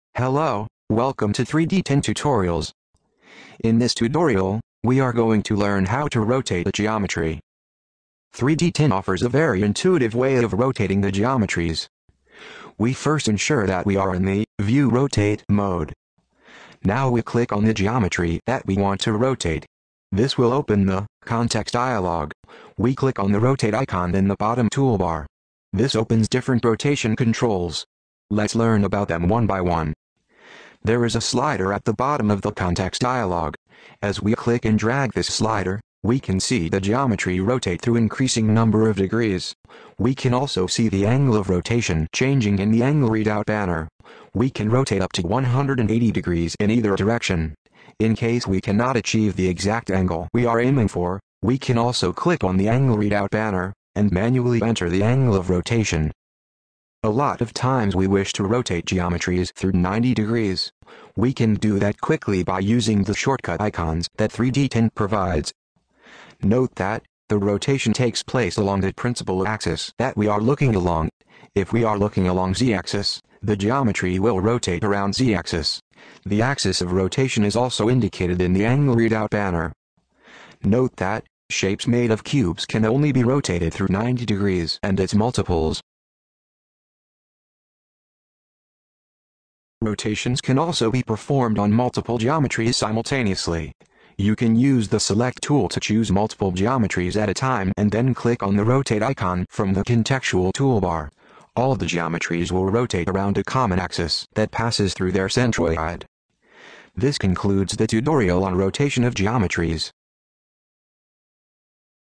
Rotating Geometries - 3DTin Tutorial - Intermediate
In this tutorial we learn how to rotate geometries in 3DTin.